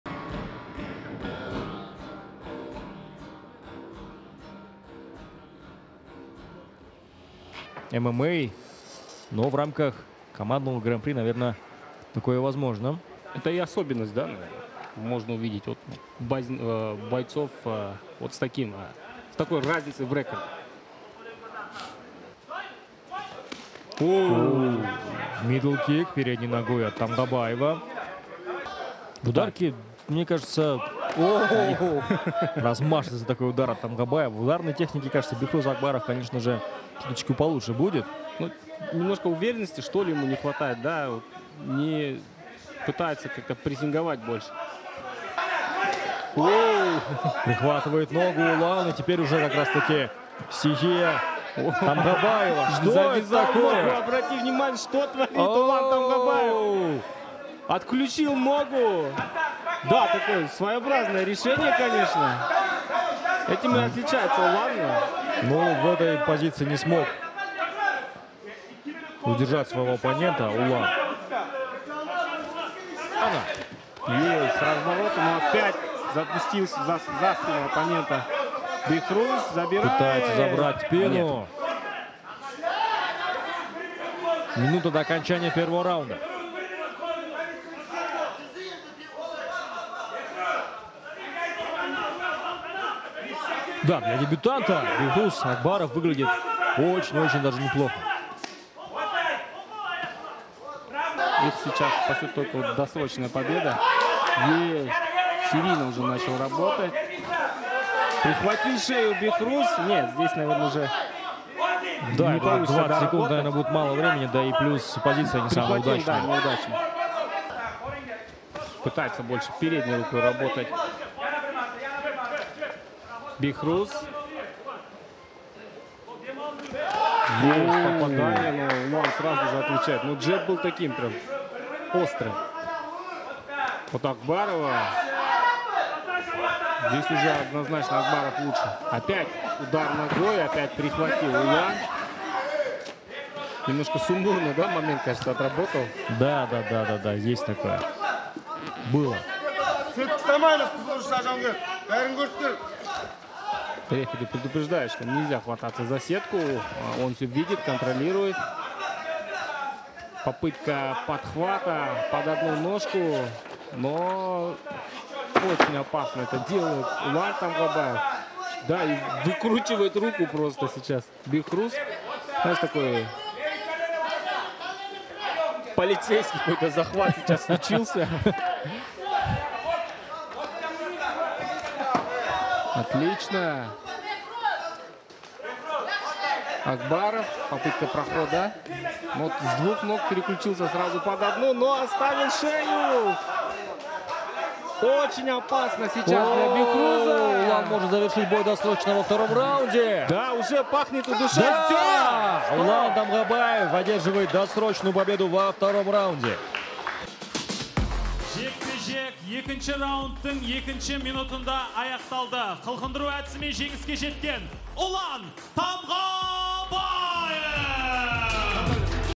NAIZA 43: ULAN TAMGABAYEV VS BEHRUZ AKBAROV
Генеральный партнер - DOSSYM.KZ. DOSSYM.KZ - ПОЛУЧИ МИКРОКРЕДИТ ПО-ДРУЖЕСКИ!

Боец DAR TEAM Улан Тамгабаев приносит команде вторую победу в гранпри!!!